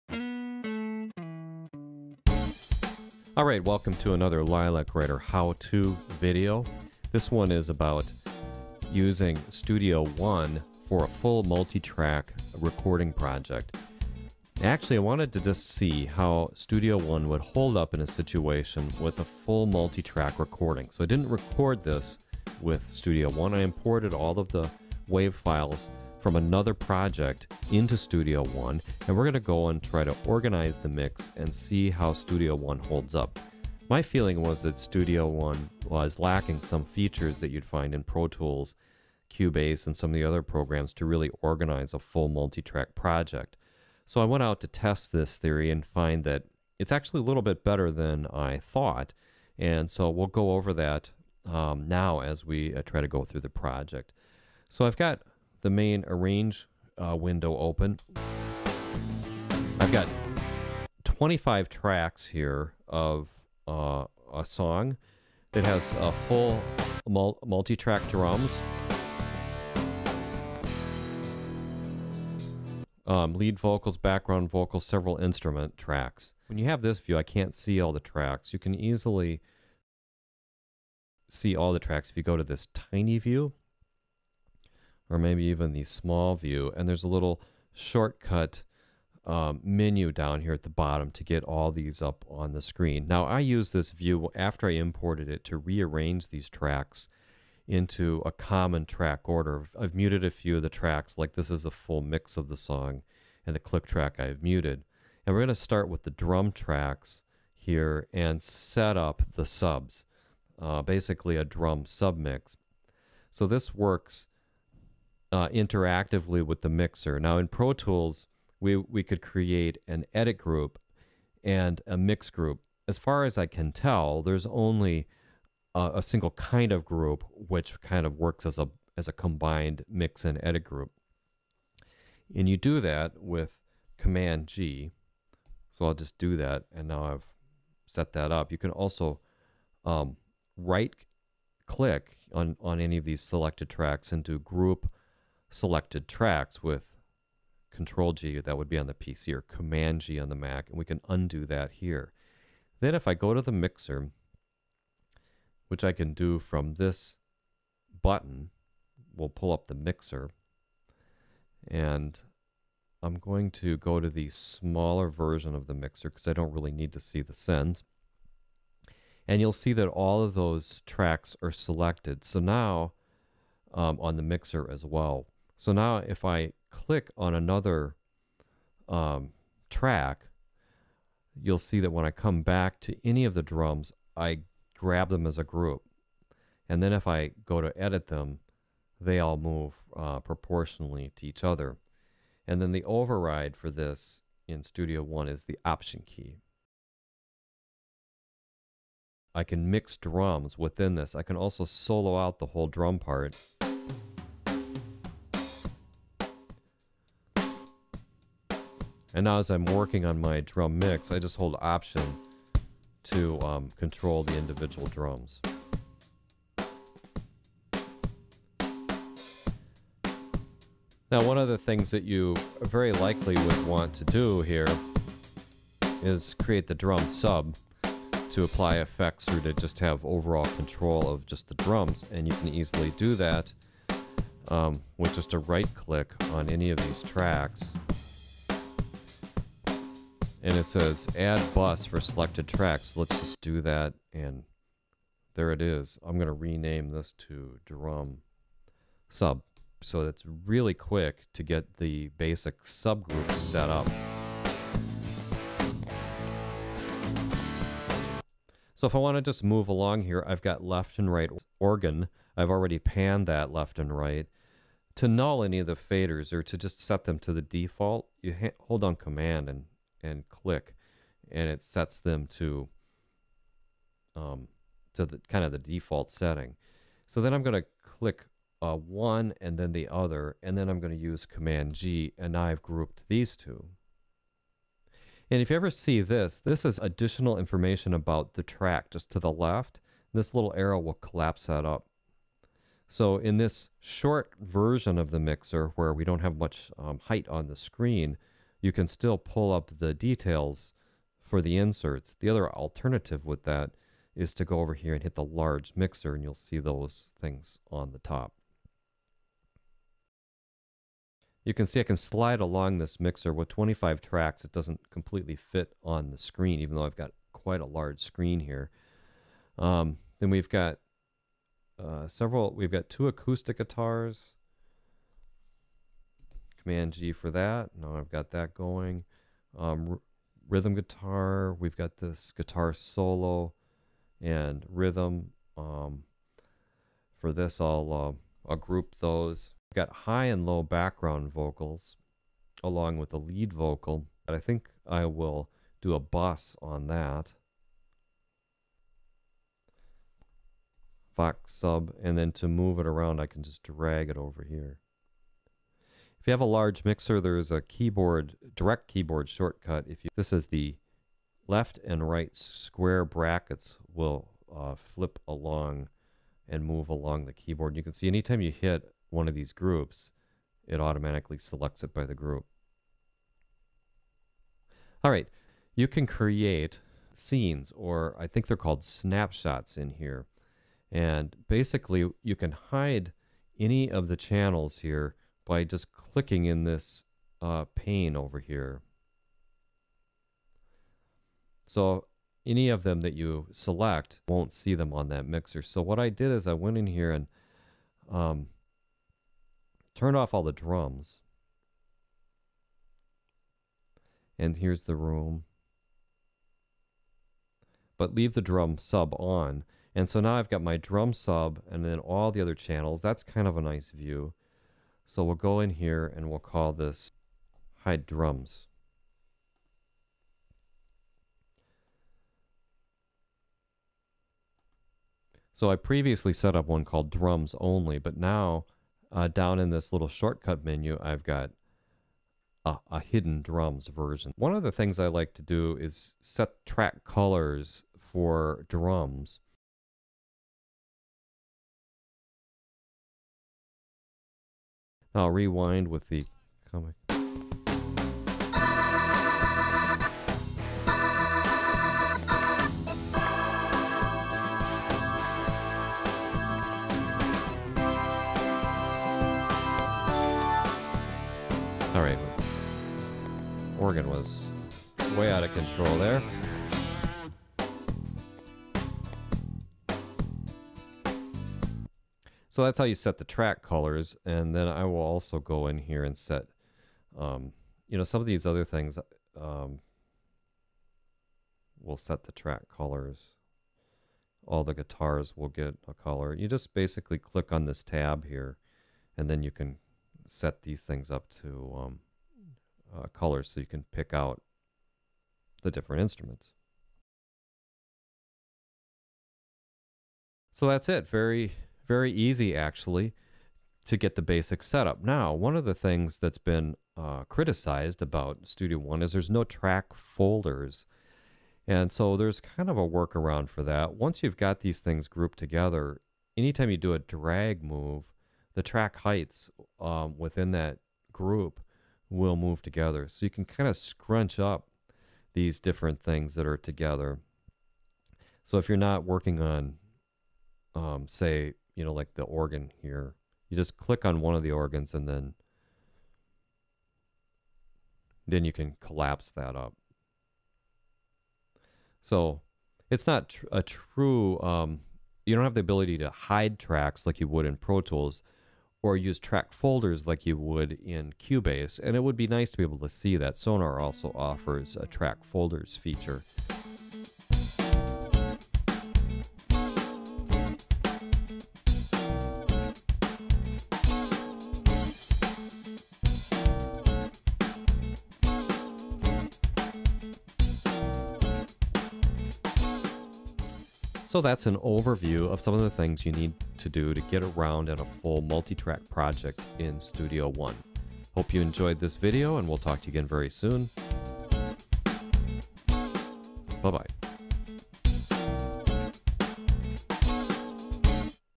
Studio One How-To: Set Up a Multitrack Mix
Studio One is a new DAW from PreSonus. I wanted to see how to set up a full multitrack mix and organize groups, track colors and subs. It turns out to be quick and easy. The main limitation is no ability to hide or collapse tracks. But this is a small inconvenience. Setting up subgroups super fast and almost a trivial compared to other popular programs.